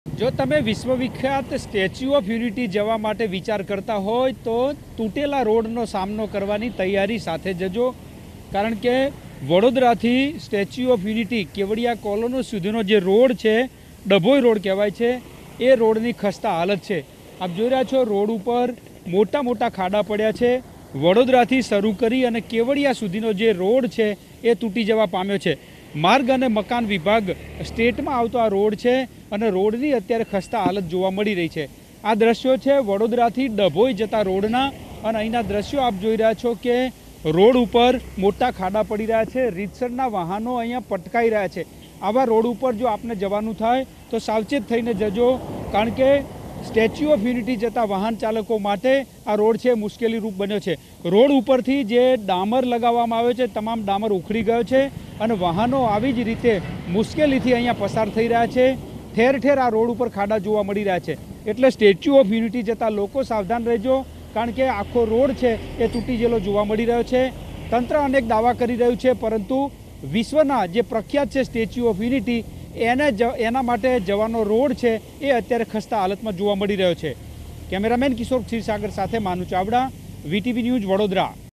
જો તમે SOU જવા માટે વિચારતા હોવ તો તૂટેલા રોડનો સામનો કરવાની તૈયારી સાથે જજો | VTV Gujarati
જો તમે SOU જવા માટે વિચારતા હોવ તો તૂટેલા રોડનો સામનો કરવાની તૈયારી સાથે જજો

વરસાદ બાદ વડોદરામાં ખરાબ રસ્તાઓની મુશ્કેલી. વડોદરા-ડભોઇ રોડ પર ખાડા પડ્યા છે. સ્ટેચ્યૂ ઓફ યુનિટી જવાનો મુખ્ય રોડ ડિસ્કો રોડ બન્યો છે. વડોદરાથી તમામ તાલુકા મથકોને જોડતા રોડ બન્યા બિસ્માર.